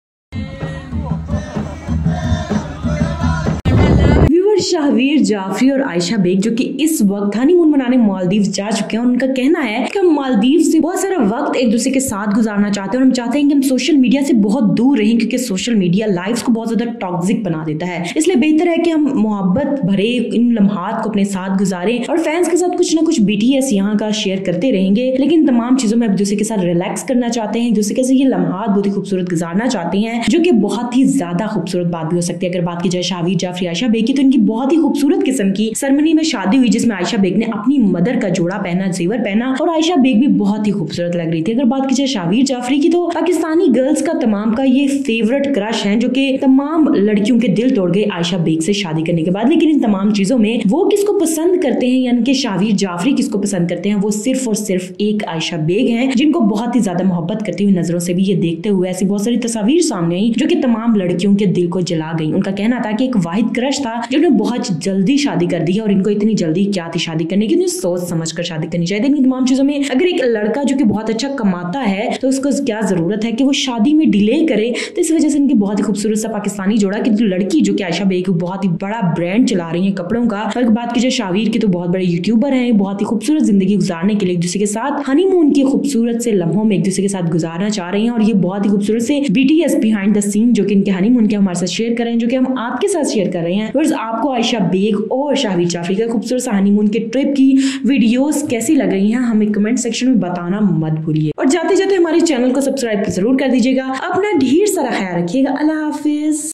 Shahveer jafry and Ayesha Beig Honeymoon at Maldives
Shahveer jafry and Ayesha beig after wedding first Honeymoon at maldives.

Showbiz HTv